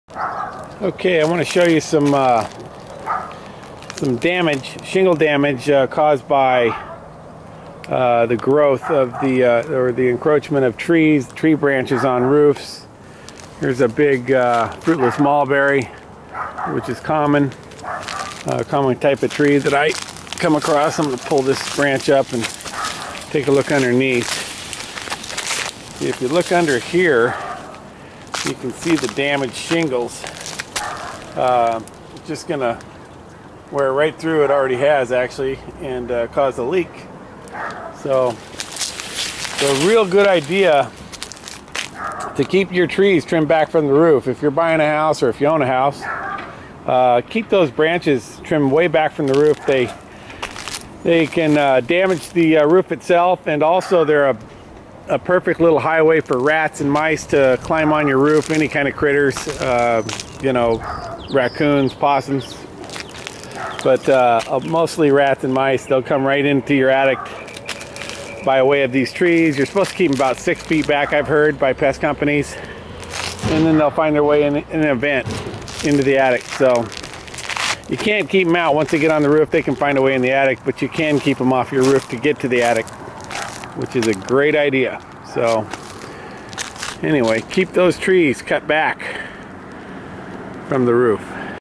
Roofing Contractors | Roofers | Dublin Ca California
Adam Wilfley | Roof Doctors | describes a composition roof and damage to the composition shingles that have been caused by overhanging tree branches and the importance of keeping tree branches trimmed on a home in Dublin California .